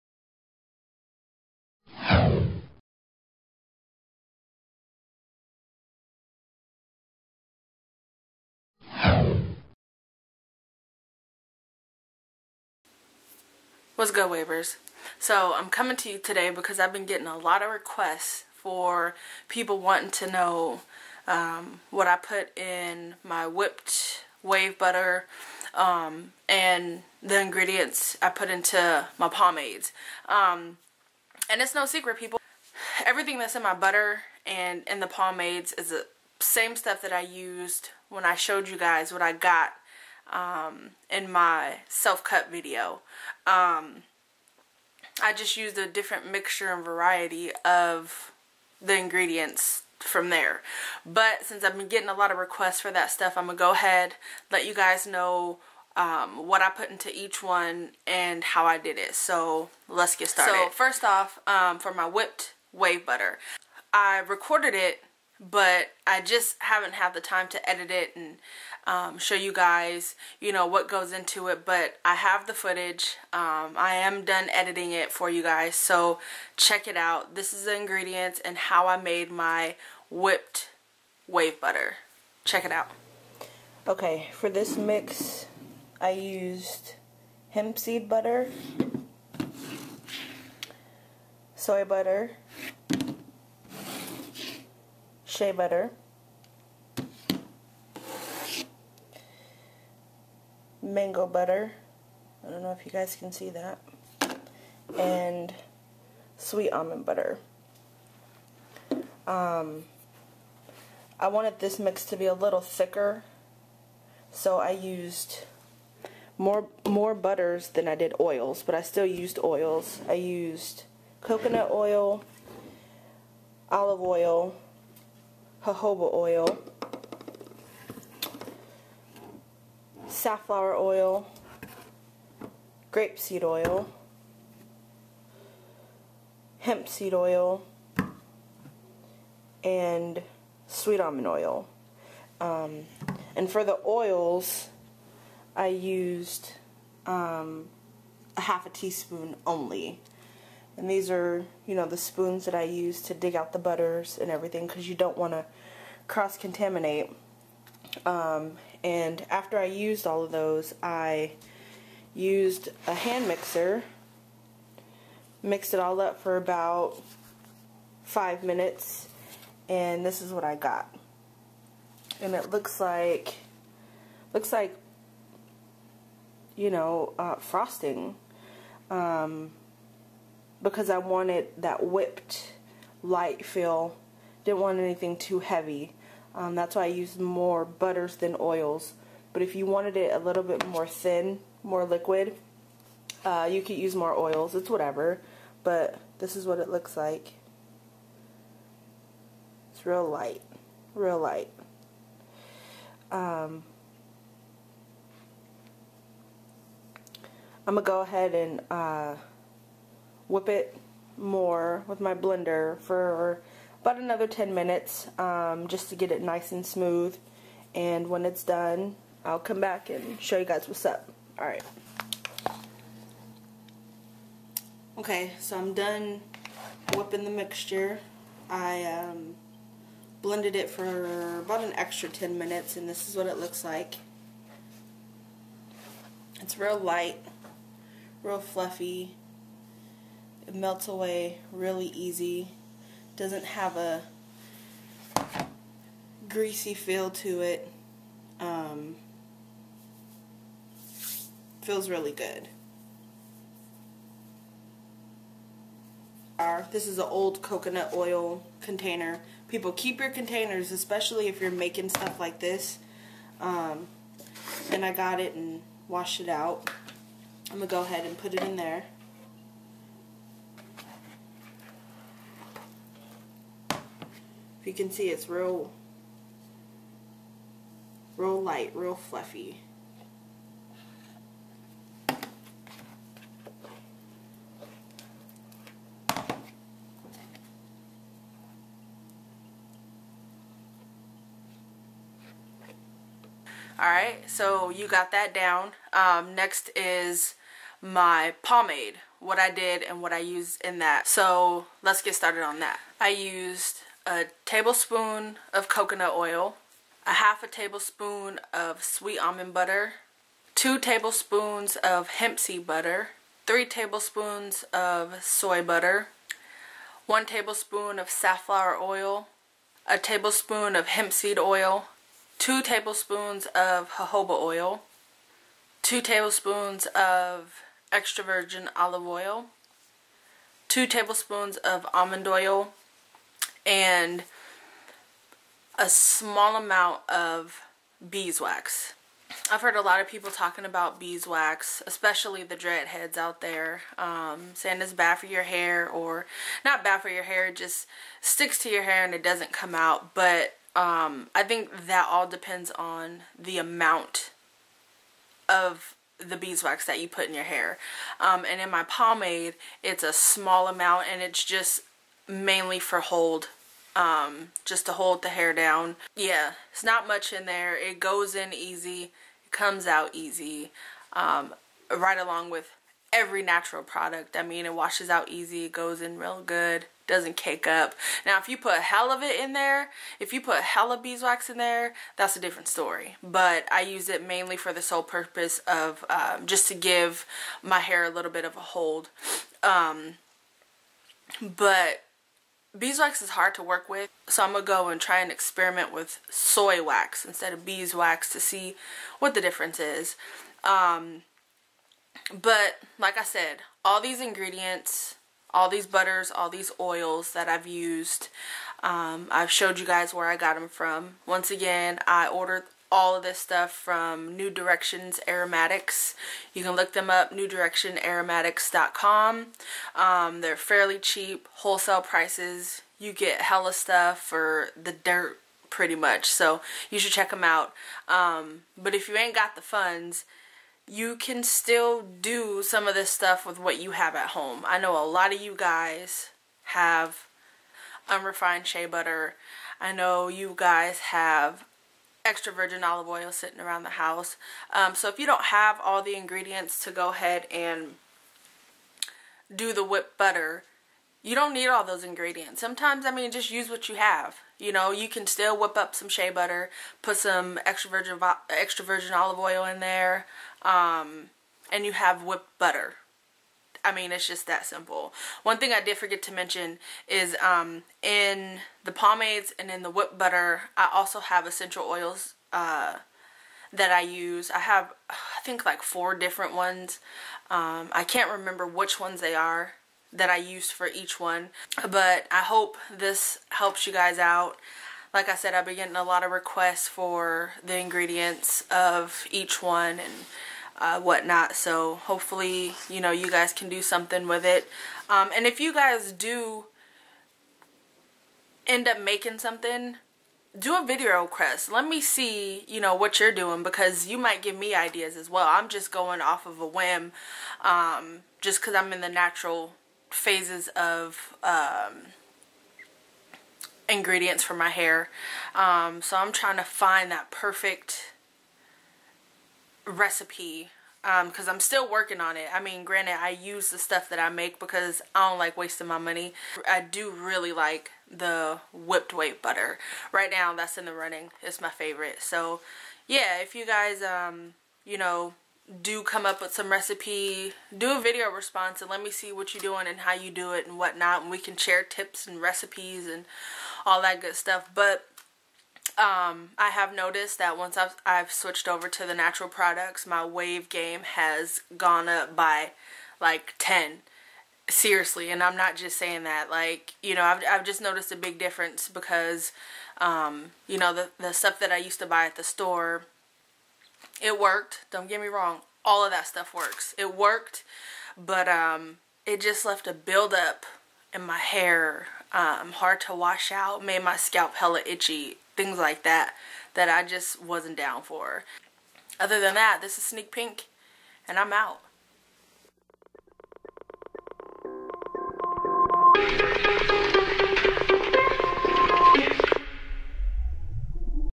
Pomade & Whipped Wave Butter Ingredients
A lot of requests for the ingredients I used for my pomade and whipped wave butter so...here you go!